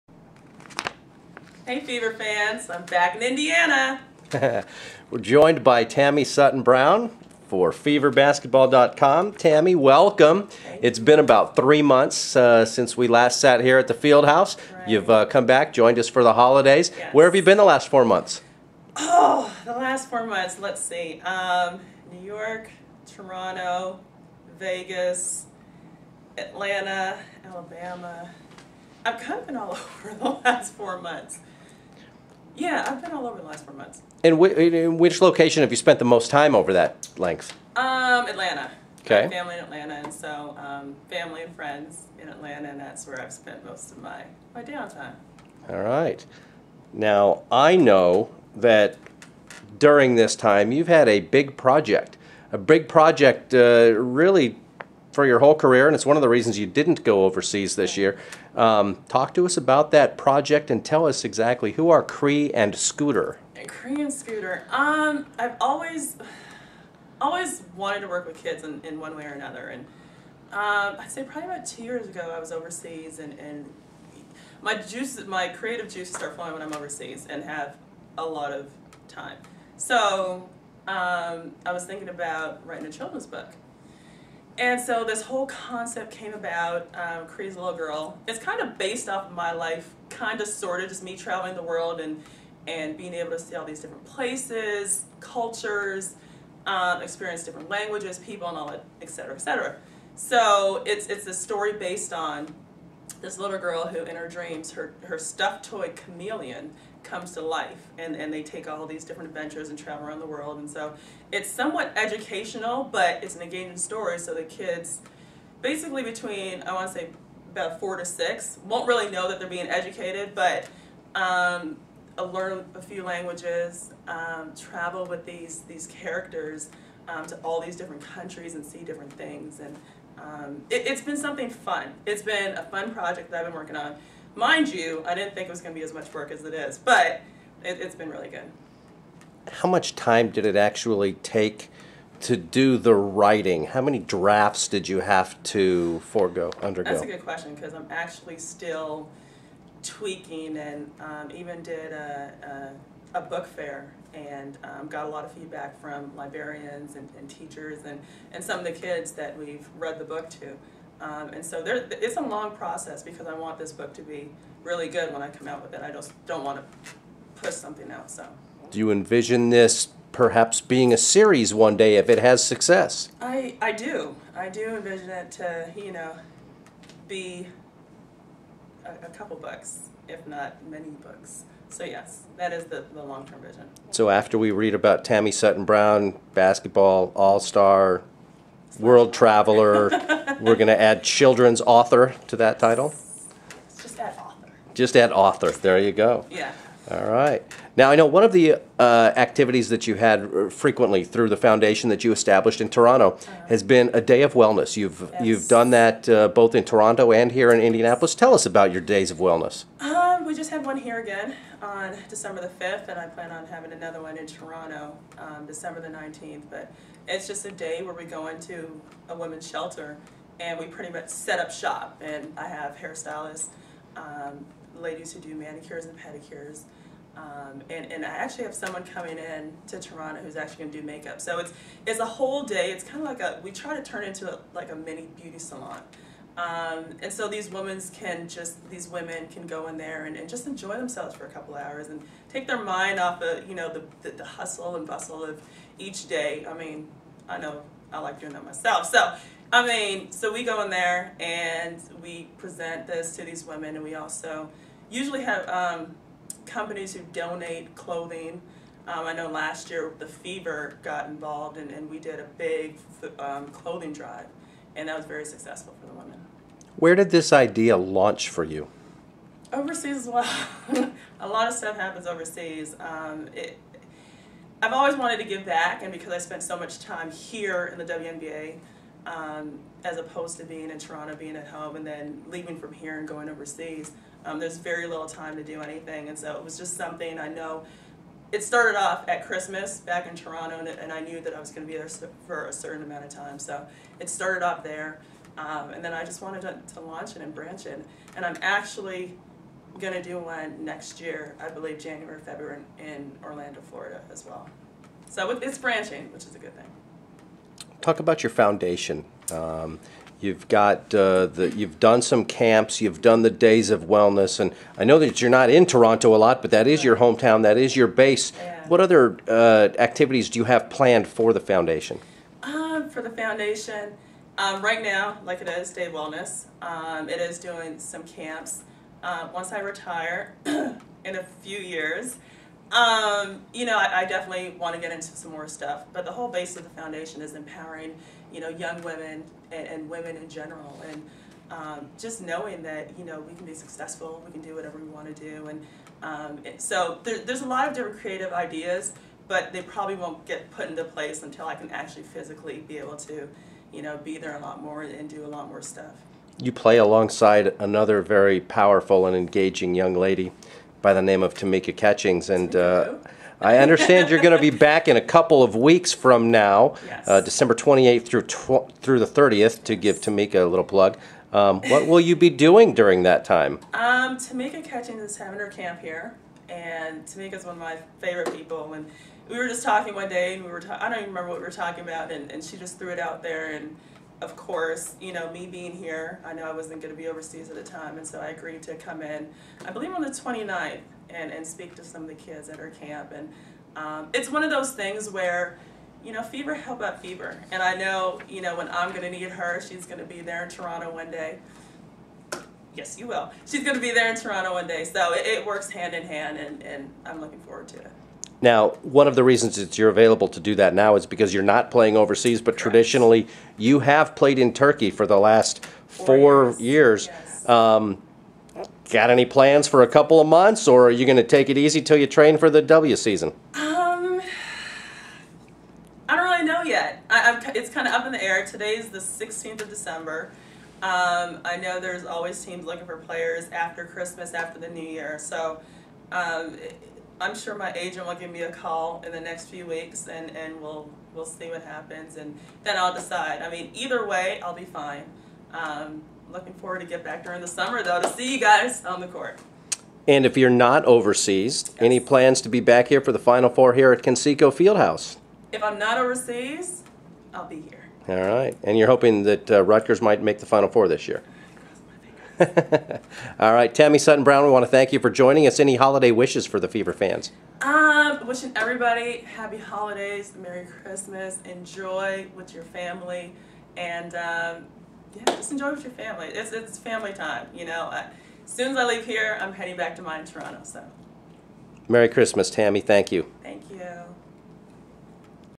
Tammy Sutton-Brown Checks In
Tammy Sutton-Brown checks in with Feverbasketball.com.